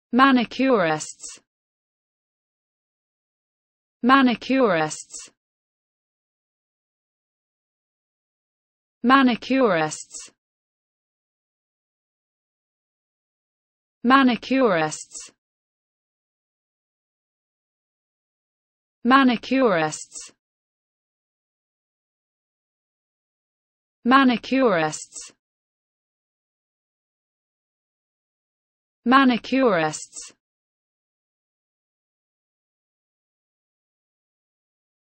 the correct pronunciation of manifolding in English.
This video shows you how to pronunciation manifolding in English.